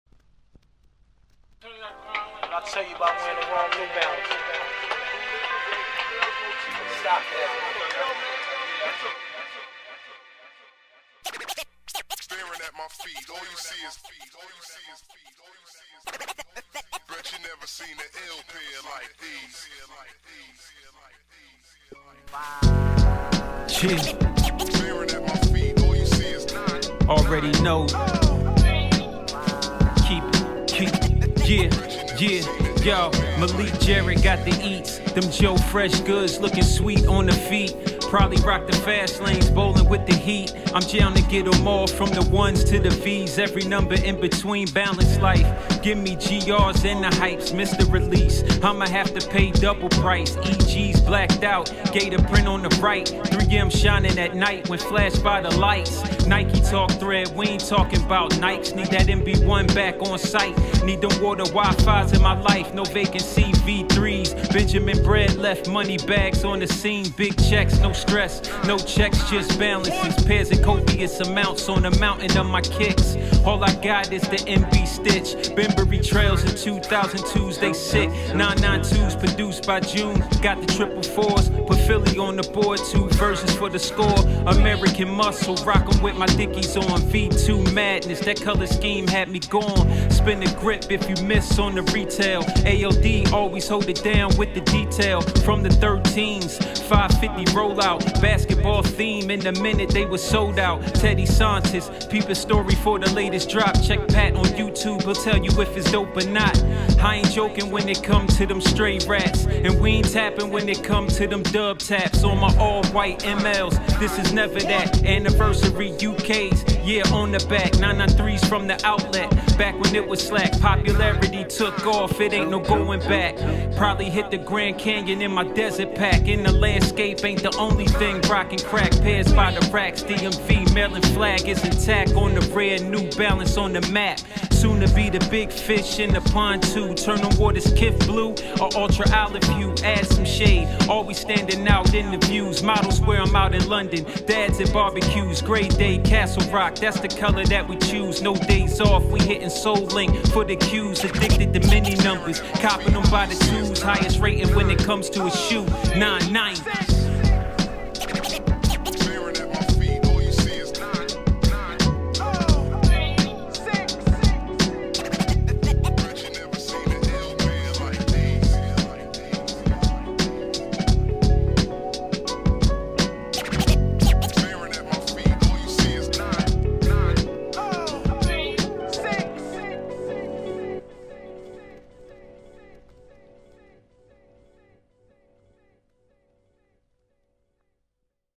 New Balance Life
A tribute to my favorite brand New Balance and the collaborators throughout the years.